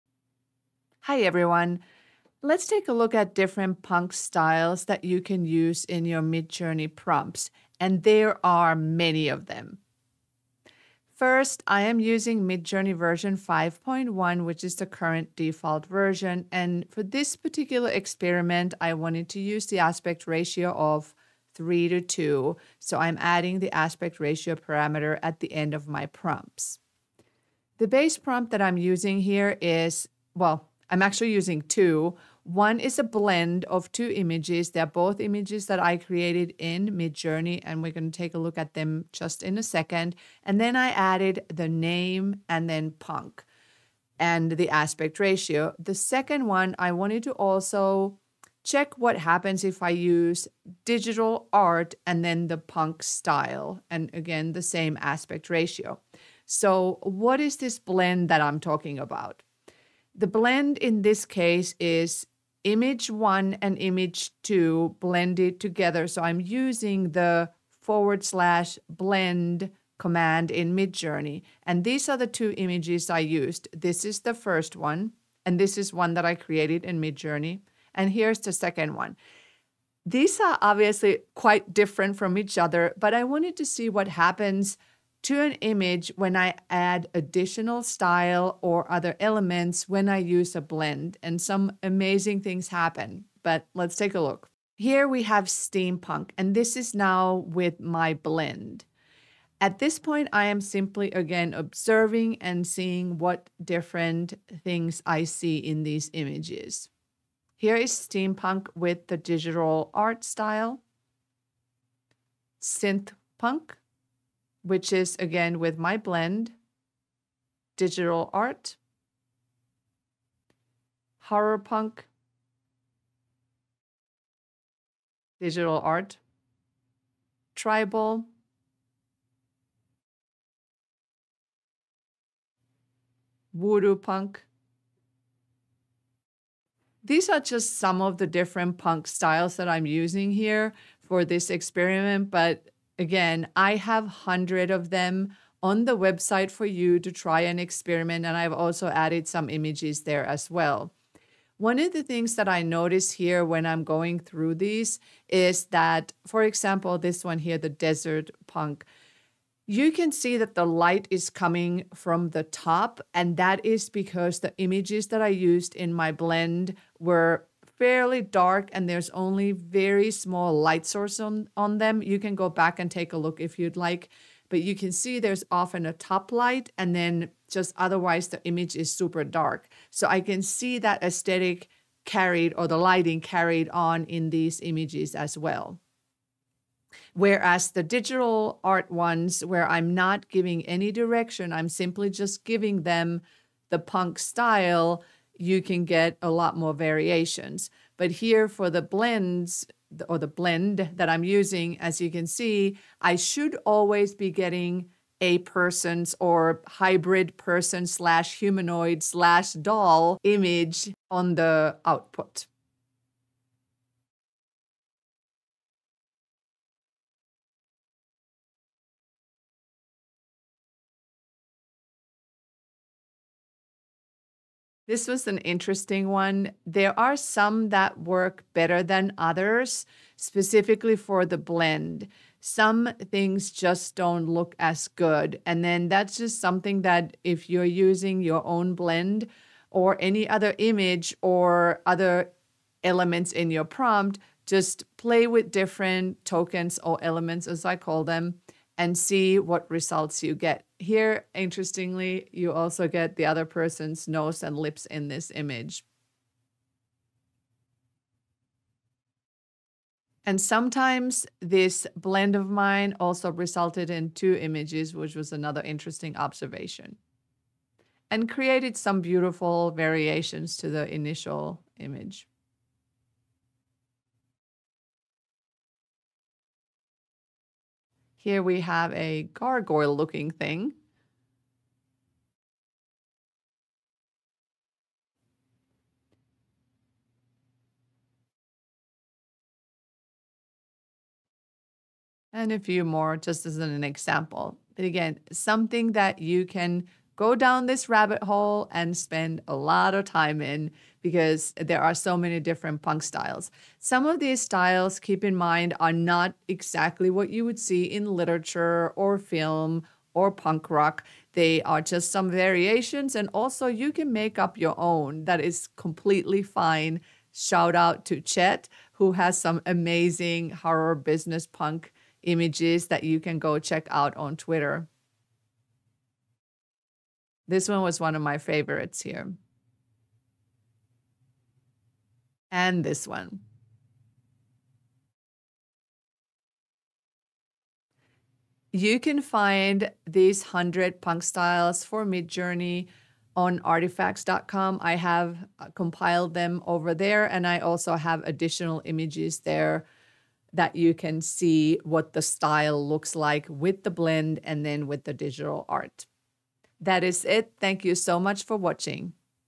Punk Styles for Midjourney Prompts - Tutorial, Beginner Friendly
Punk Styles for Midjourney

Punk subculture can be found in music – punk rock, literature, film, art, dance, fashion, and you can even make up your own punk words.

These punk genres can add interesting and quite specific aesthetics to your prompts, depending on the “punk” you choose.

Here’s a walk-through of a few punk styles. Keep in mind that Midjourney doesn’t necessarily interpret the style as it’s described in punk rock, etc.

Some of the punk styles give a stronger aesthetic than others, and a few are more challenging for Midjourney to interpret. Consider adding other descriptors to your prompts to help guide Midjourney more to your desired result.

Prompts I used in this video:

URL 1 + URL 2 (my blend) [style]punk --ar 3:2
and

[style]punk, digital art --ar 3:2

For example:
steampunk, digital art --ar 3:2
voodoopunk, digital art --ar 3:2
tribalpunk, digital art --ar 3:2

These are simple base prompts to observe the effects of the punk style. You can then add to the prompt based on what you see or per your preferences.

Shoutout to Chet for his Horror Business Punk creations!! at: chetbff on twitter.